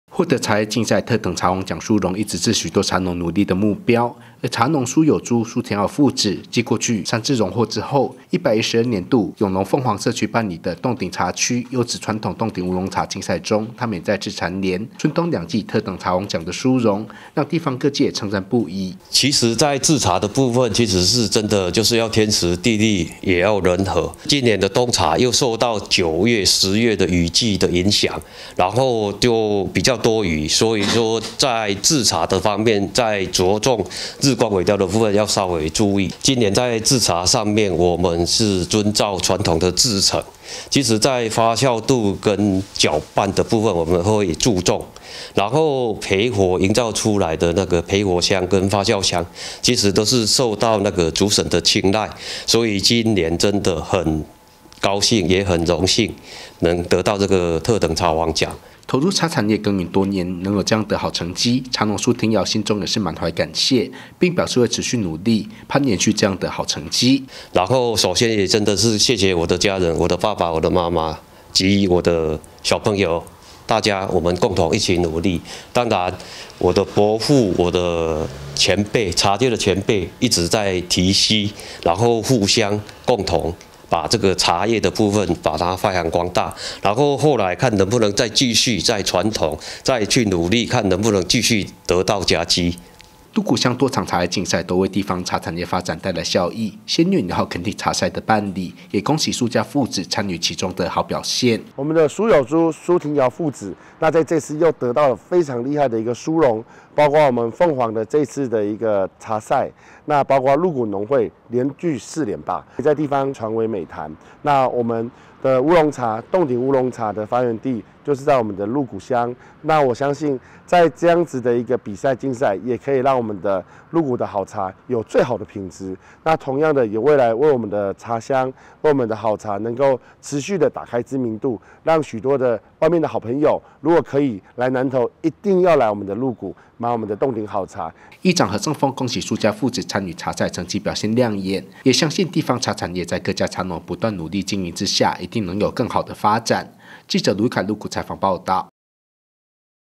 20240109 南投縣議會 民議新聞 蘇家父子蟬聯特等茶王獎 成為茶農效法典範之一(縣議員 游顥)
茶農蘇有朱、蘇庭瑤父子，過去就曾榮獲三次特等茶王獎殊榮，而112年永隆鳳凰社區辦理的凍頂茶區優質傳統烏龍茶競賽中，他們也再次榮獲春冬兩季特等茶王獎，成為許多茶農學習效法目標。議長何勝豐恭喜蘇家父子參與茶賽成績表現亮眼，也相信地方茶產業，在各家茶農不斷努力經營下，一定能有更好的發展。